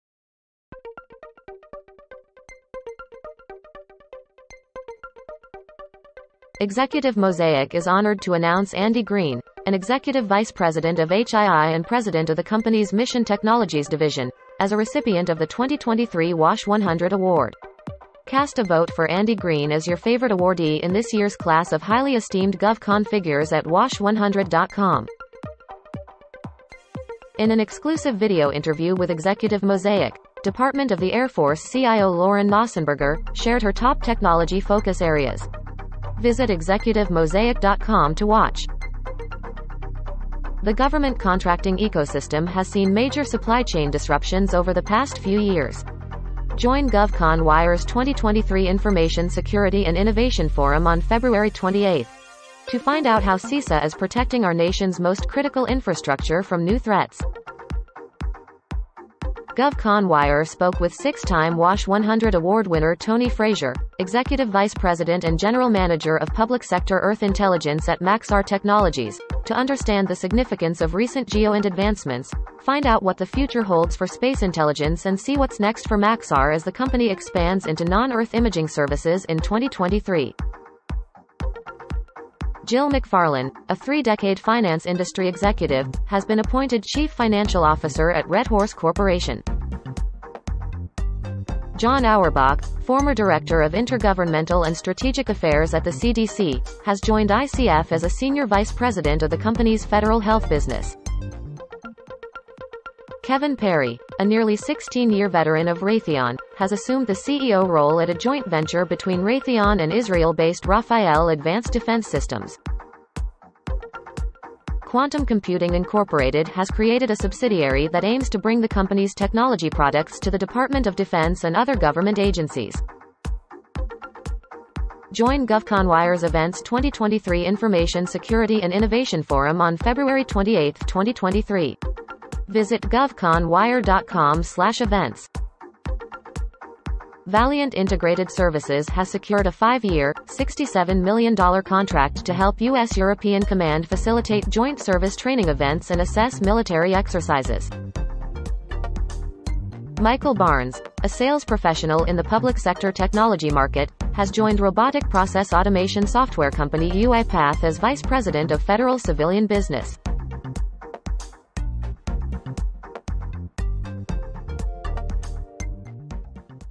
Executive Mosaic Video: Top Stories from GovCon Wire 2/7/2023
✅ Below are the Top GovCon Stories from govconwire.com on 2/7/2023

00:06  Andy Green
Andy Green, HII Mission Technologies President, Named to 2023 Wash100 for Leadership in Portfolio Transformation & Business Growth Initiatives

00:26  Lauren Knausenberger
Video Interview: Air Force CIO Lauren Knausenberger Shares Top Tech Focus Areas

00:40  Cybersecurity and Infrastructure Security Agency
How CISA Is Tackling Supply Chain Issues & Rising Cyber Threats

00:59  Tony Frazier
Maxar’s Tony Frazier Shares How 3D & 4D Data is Changing GEOINT

01:21  Jill McFarlane
Jill McFarlane Named Redhorse CFO; John Zangardi Quoted

01:31  John Auerbach
ICF Appoints Former CDC Official John Auerbach as Federal Health Business SVP

01:42 Kevin Perry
Kevin Perry Takes Helm of Raytheon-Rafael Joint Venture

01:54 Quantum Computing Inc.
Quantum Tech Maker QCI Forms Government-Focused Business; Robert Liscouski Quoted

02:04 Information Security and Innovation Forum

02:16 Valiant Integrated Services
Valiant to Support US Military Joint Training Program Under $67M Contract; Dan Corbett Quoted

02:29 Michael Barnes
UiPath Hires Michael Barnes as Federal Civilian VP